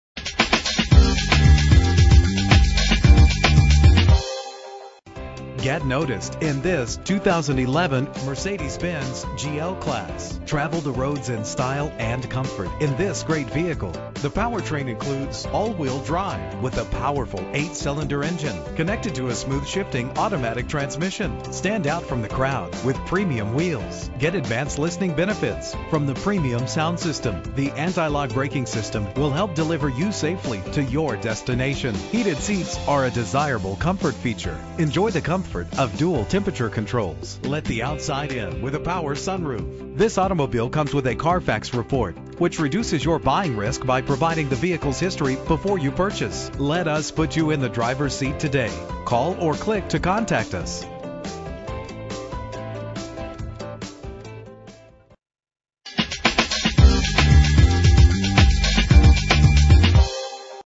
2011 Mercedes-Benz GL-Class - Akron OH
Year: 2011
Make: Mercedes-Benz
Model: GL-Class
Trim: GL450 4MATIC
Engine: 4.6 liter 8 cylinder 32 valve
Transmission: 7-Speed Automatic
Color: Arctic White
Mileage: 28709
Address:
1345 E Market Street